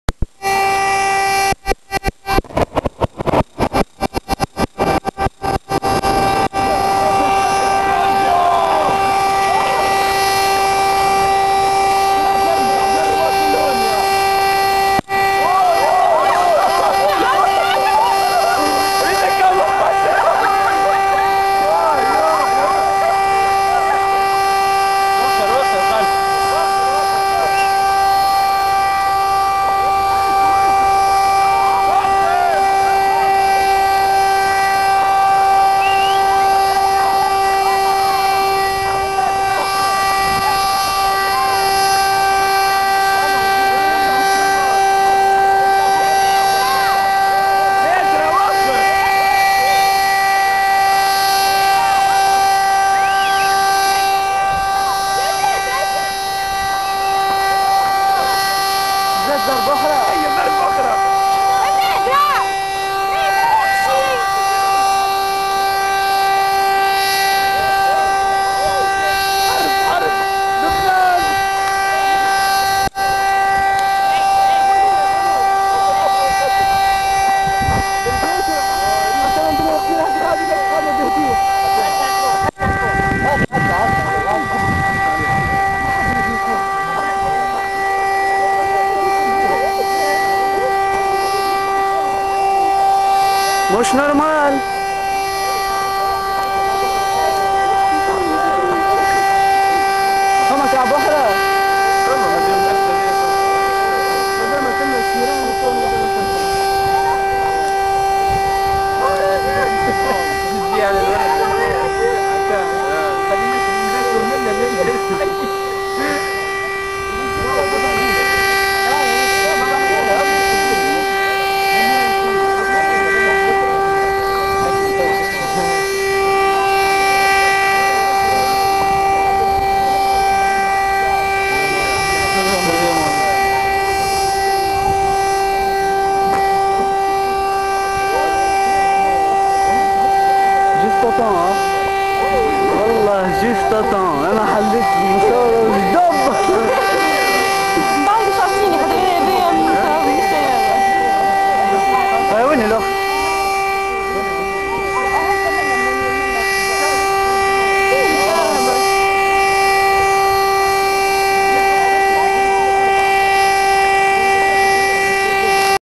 explosion a djebel reças(st2 sortie)1
la 2eme viddeo d'explosion de dynamite
a djebel reças
lors de la sortie st2(que du bonheur)